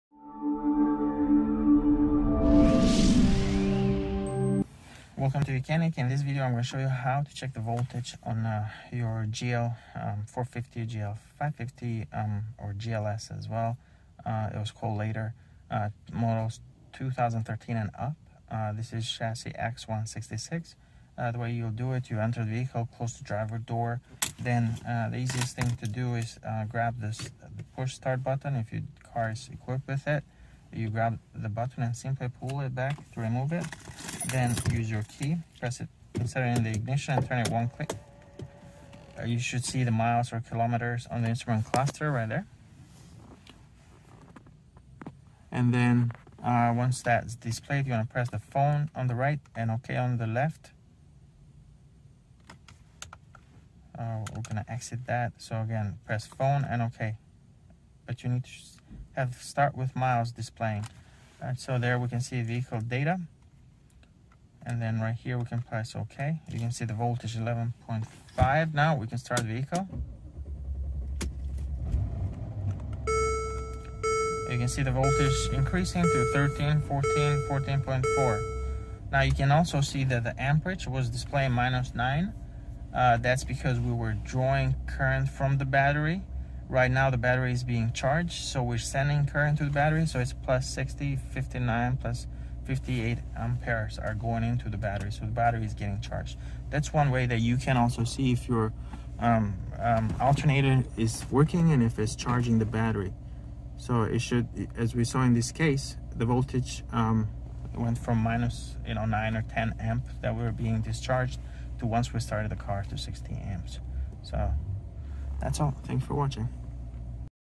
How to Check Voltage on Mercedes Benz GL & GLS Class Instrument Cluster
This video provides a comprehensive guide on how to check the voltage on the instrument cluster of a Mercedes Benz GL & GLS Class vehicle. The tutorial provides step-by-step instructions on how to test the voltage output from the instrument cluster using a multimeter, which can help diagnose any electrical issues affecting the cluster's functioning. The video also covers important safety tips and procedures to ensure the testing is done safely and effectively. The tutorial is clear and easy to follow, with helpful visuals to guide viewers through the process. By watching this video, Mercedes-Benz owners can save money on dealership repairs and diagnose any electrical issues affecting their vehicle's instrument cluster for optimal performance and safety.